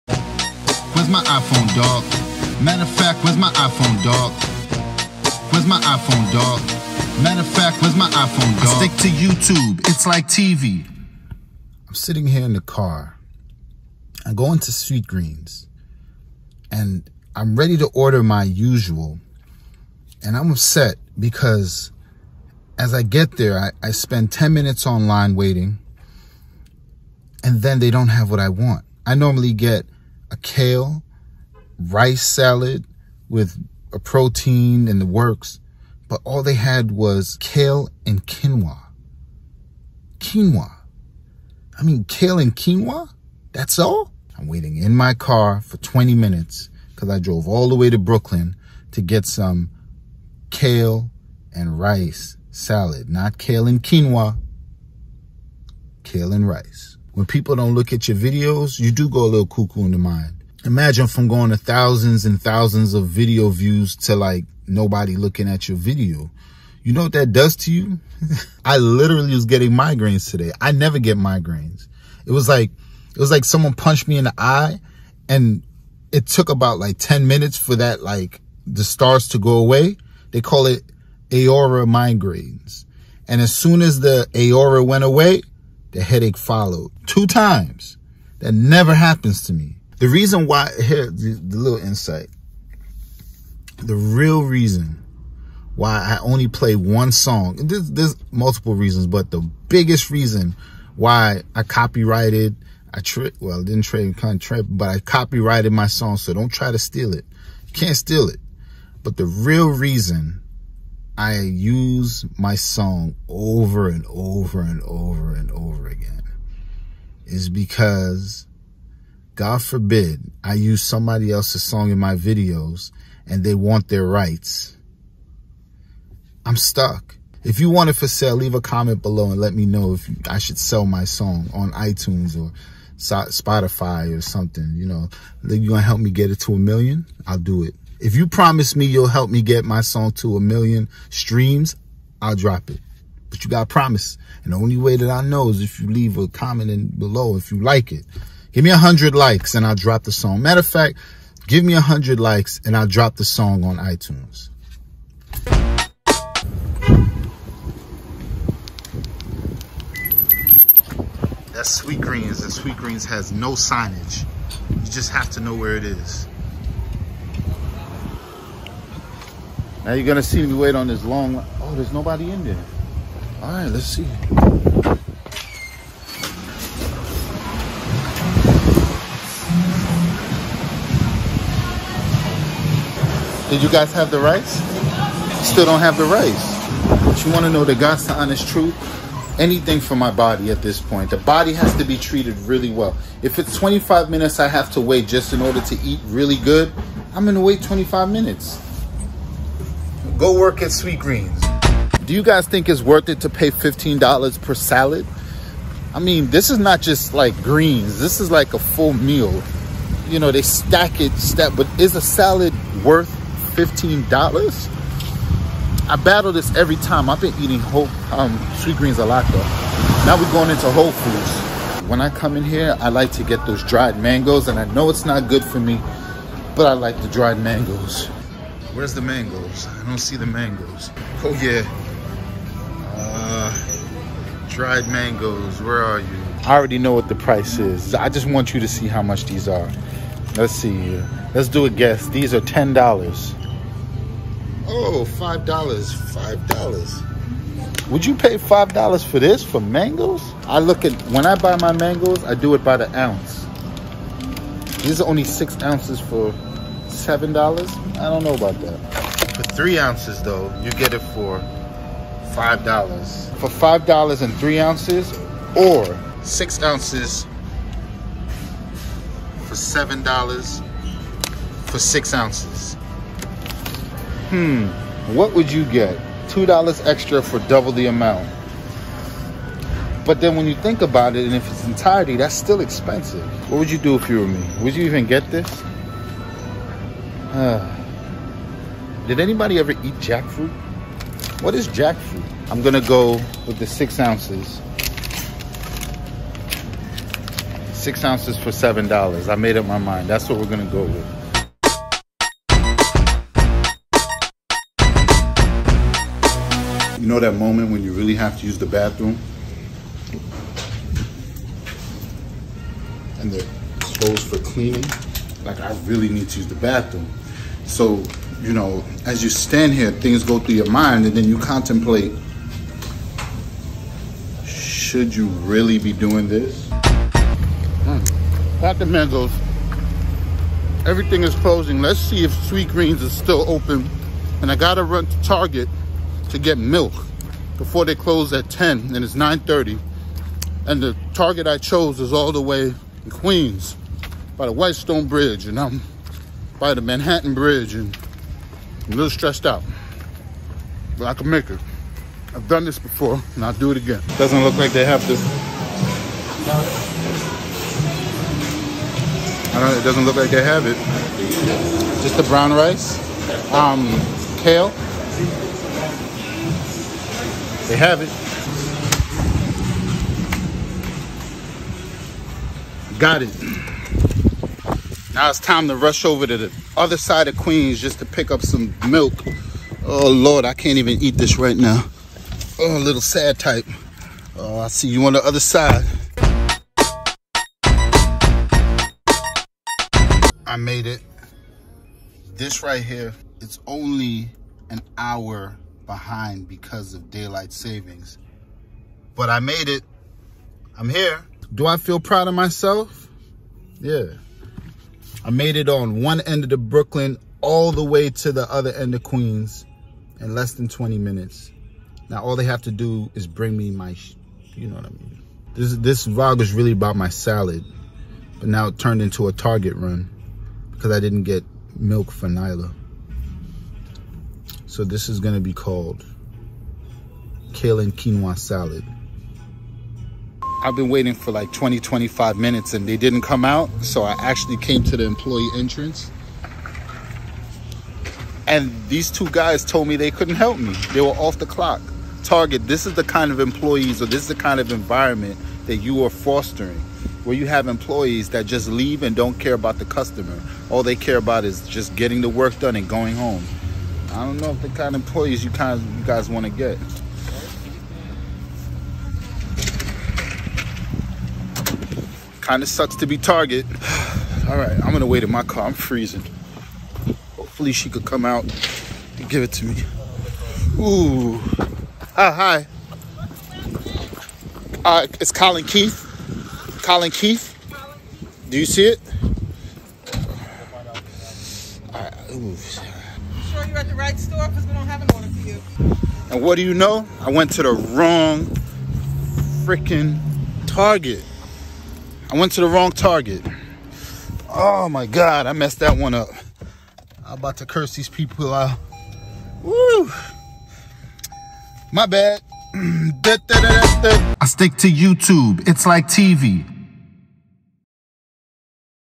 not eating sweetgreen’s kale and quinoa salad!
sweetgreen kale and quinoa salad tastes like whole food frozen mixed vegetables! i made the mistake of eating sweetgreens' kale and quinoa salad and I wasn't about to make that same mistake again.

i went in for my sweetgreens salad- kale, brown rice, tomatoes, blackend chicken, etc, and they didn't have brown rice but they had quinoa. i asked them how long it would take and they told me it would take 20 mins. right before this, i ordered whole milk for nyla at target all the way on the other side of queens, and i was on the opposite end of brooklyn. it was 9pm and target closes at 10 and i was in a bit of a rush.

I waited 25 mins before leaving to go to wholefoods to get my favorite dried mangoes and when i got to where i needed to, i realized the mangoes are outrageously priced. i was in a dilemma: buy the overpriced mangoes or not. i decided to buy the overpriced mangoes. on a side note, whole foods is extremely overpriced. i thought amazon was going to eventually bring down the cost, but i guess that would make for bad business.

when i got back to sweengreens, my server had an attitude, probably because i made them make a whole new batch of rice and it was probably going to waster after, but hey...that's the cot of doing business. he makes me a struggle salad but i was so hungry, i didn't even bother with telling him how poor a performance his scoop game is.

with 20 mins to go, i zipped across brooklyn to queens and with 5 mins left, i finally made it to target on the other side of queens to only find out i was at the wrong target. FAIL!

Send $1 instead of hitting the "like" button